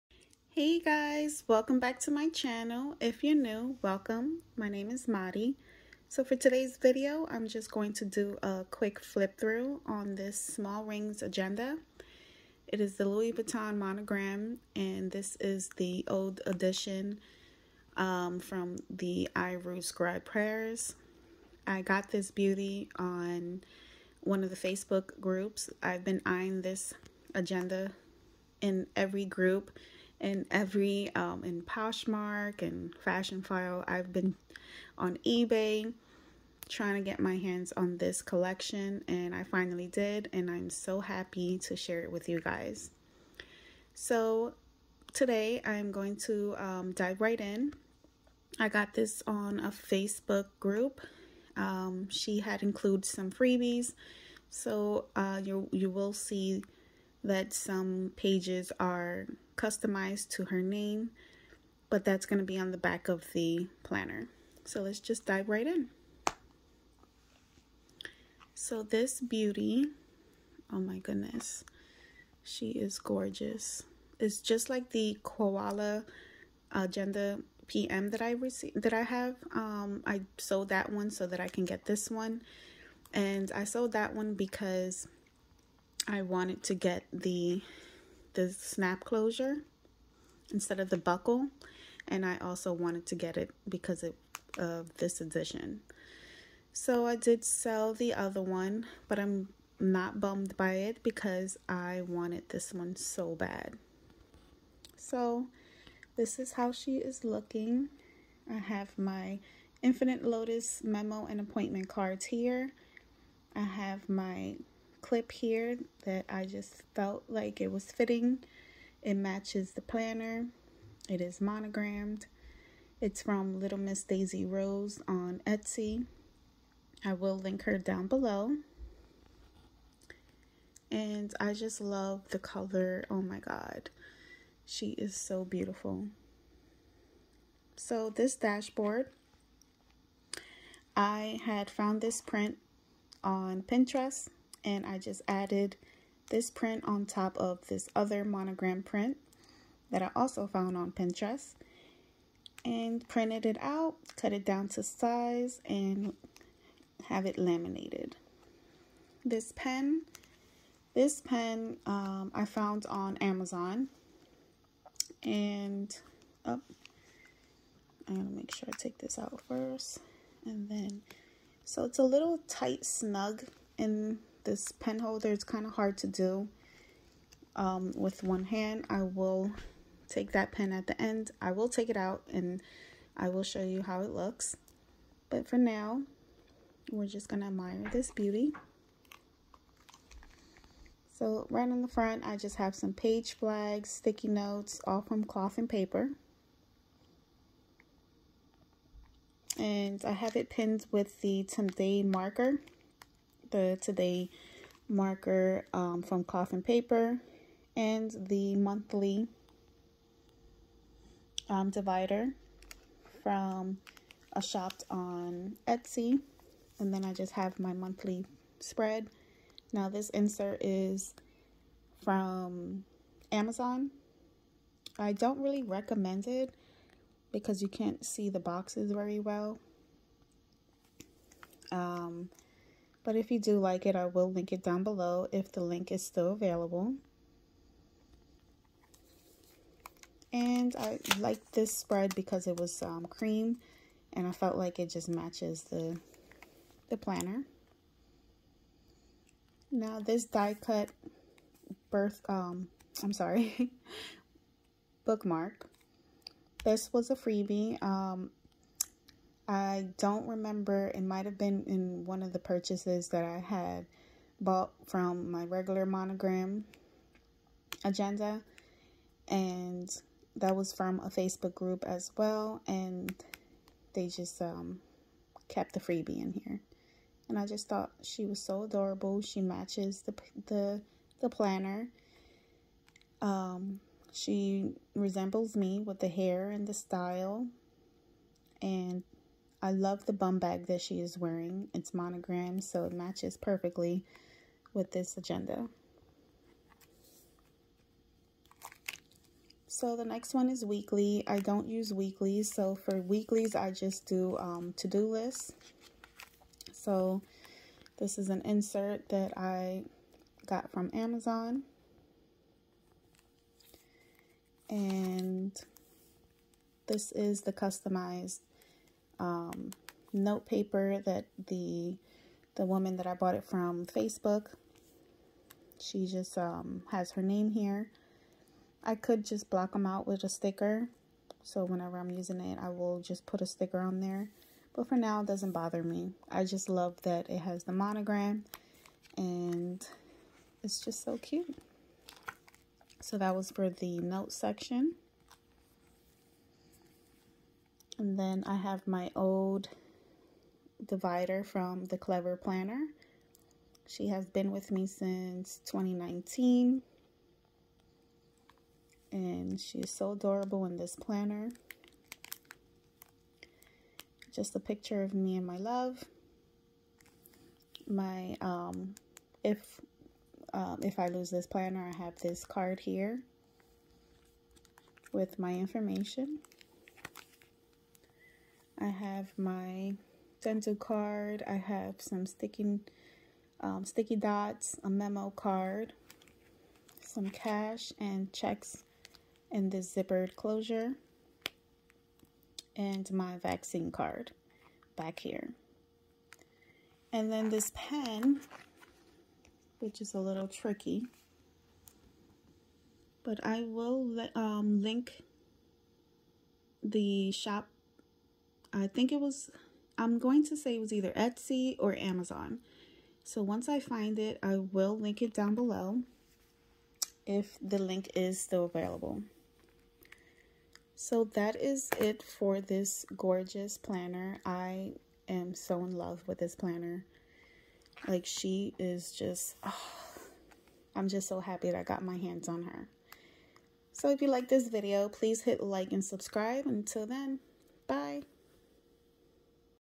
Louis Vuitton | Monogram | small rings agenda | I Rue Scribe Paris collection | flip through
Memo sticky notes/Page flags/Inbox dashboard.

Similar pen that will fit the planners.

Pilot LFBKS-1SUF-B Frixion Ink Ballpoint Pen Frixion Ball Slim Biz, Black Ink, Black.

Pink pen

Planner Refill, Pocket Notebook,...